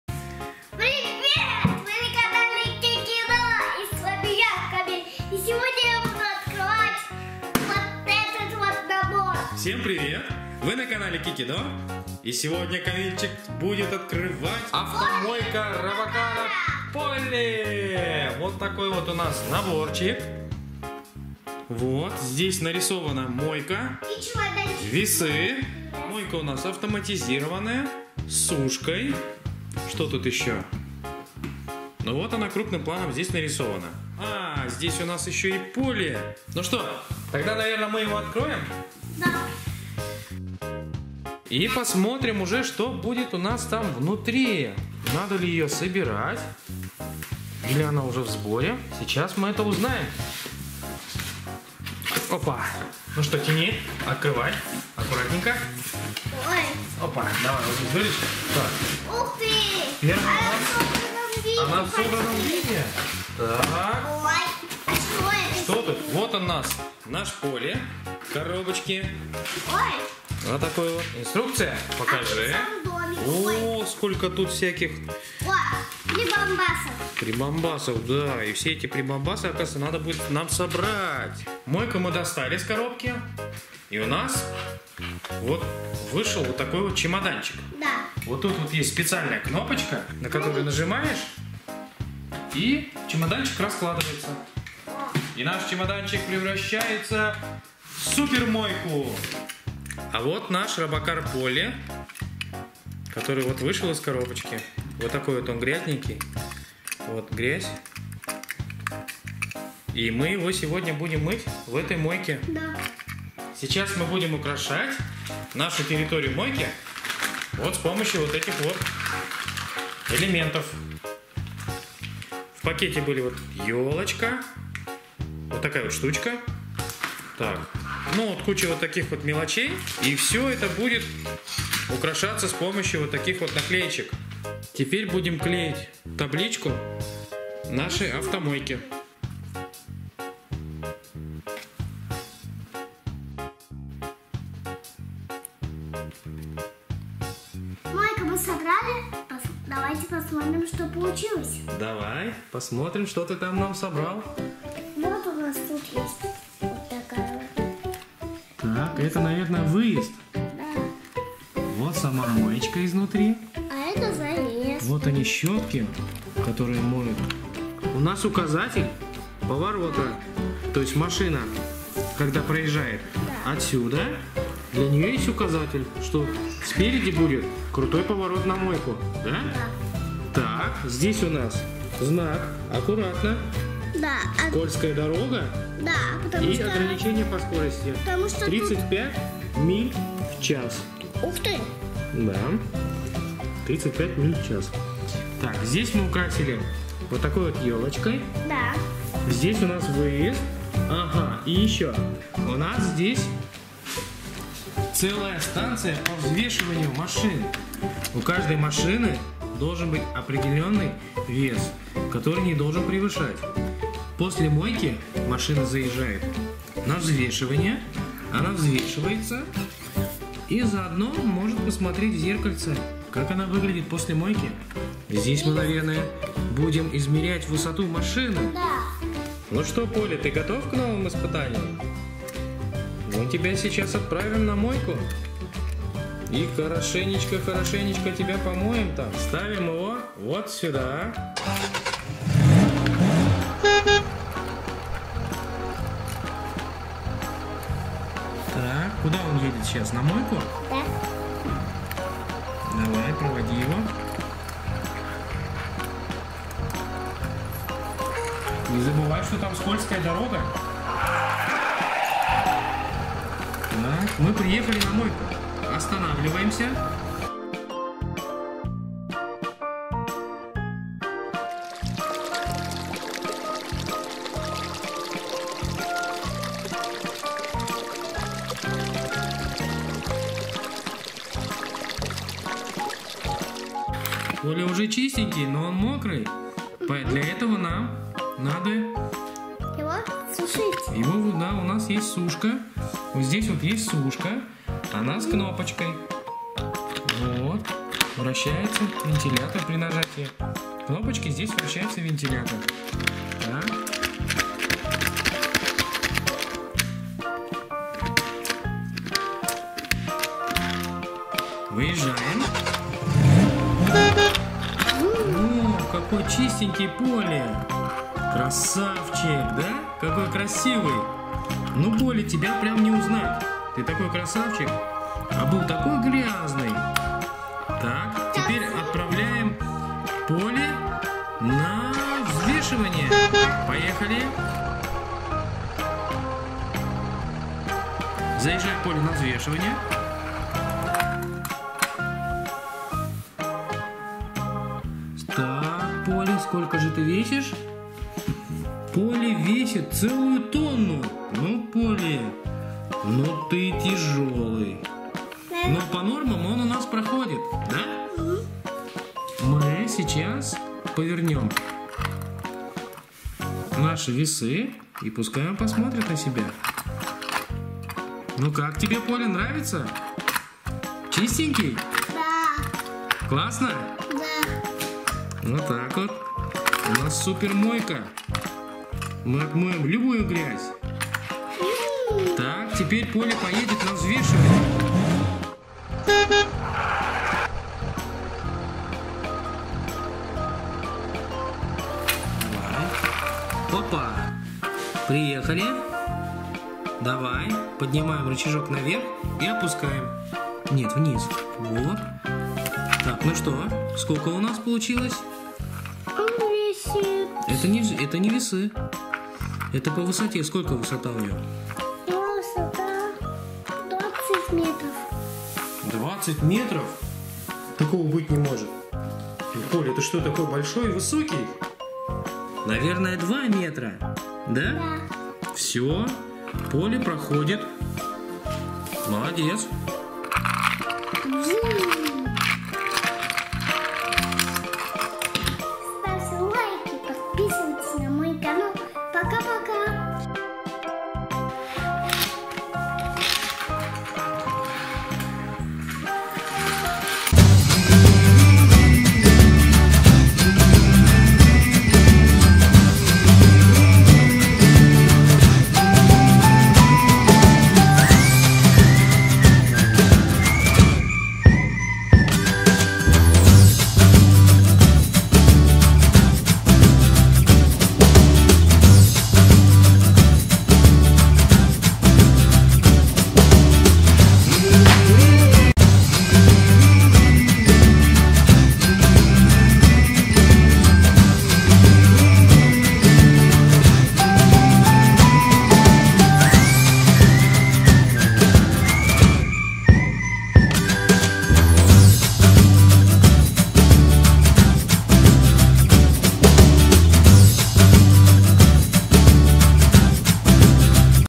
Робокар Поли Автомойка Car Wash Robocar Poli Unboxing видео для детей
Робокар Поли Автомойка Car Wash Robocar Poli. Распаковка и игра с Новом набором Робокара Поли Автомойка Робокара Поли.